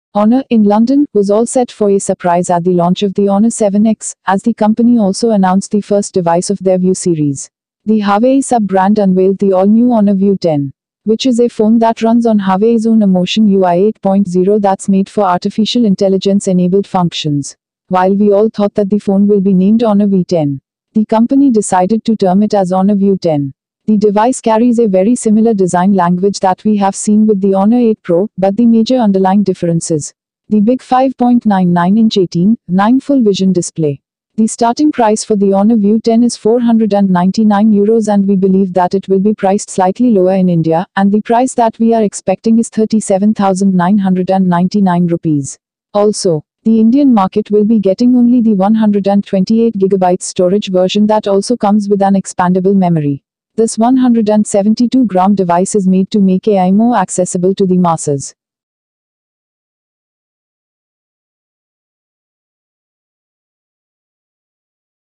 Honor View 10: The Affordable Artificial Intelligence Smartphone For 2018
Honor, in London, was all set for a surprise at the launch of the Honor 7X, as the company also announced the first device of their View series. The Huawei sub-brand unveiled the all-new Honor View 10, which is a phone that runs on Huawei’s own Emotion UI 8.0 that’s made for artificial intelligence enabled functions.

While we all thought that the phone will be named Honor V10, the company decided to term it as Honor View 10. The device carries a very similar design language that we have seen with the Honor 8 Pro, but the major underlying difference is the big 5.99-inch 18:9 full vision display. The starting price for the Honor View 10 is 499 Euros and we believe that it will be priced slightly lower in India, and the price that we are expecting is Rs 37,999. Also, the Indian market will be getting only the 128GB storage version that also comes with an expandable memory. This 172-gram device is made to make AI more accessible to the masses.